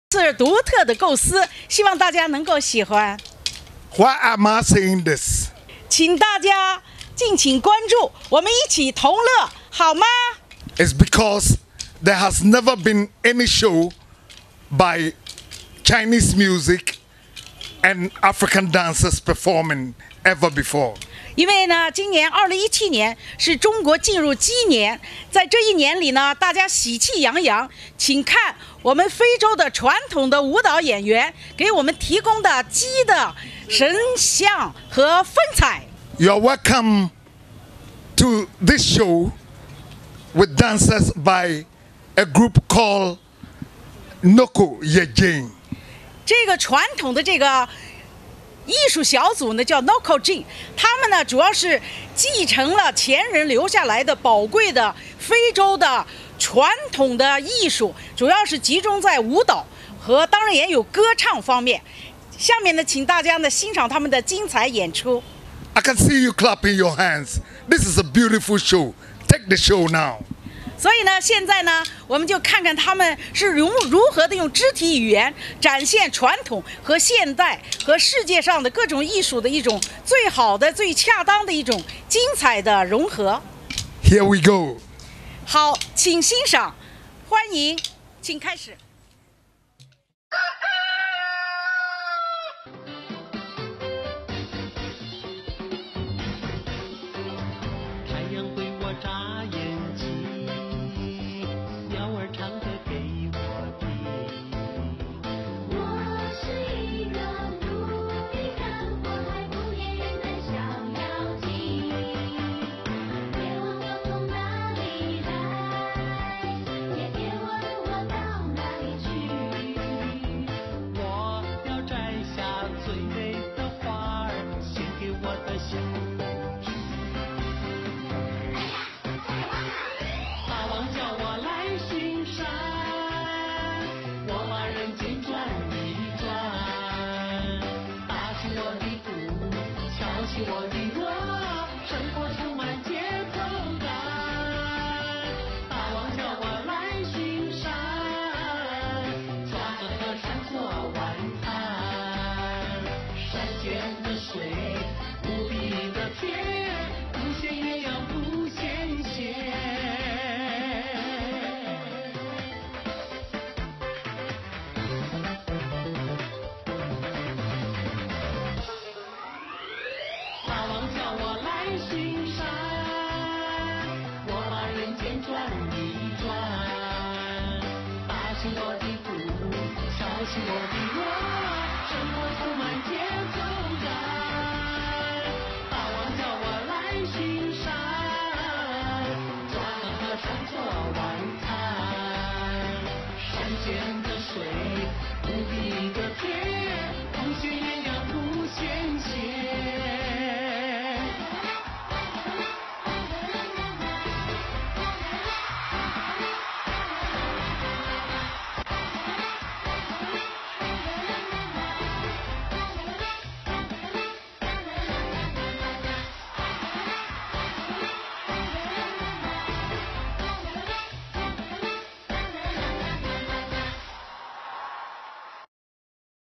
introduction Mr. 黄思伟 and his friends from Singapore, April 2017 in Accra
Dubbing by Xiaoqing Yang

Herewith one of my programs with Kwabena Boadu Anafo in Ghana, April 2017:   introduction Mr. 黄思伟 from Singapore, in Ghana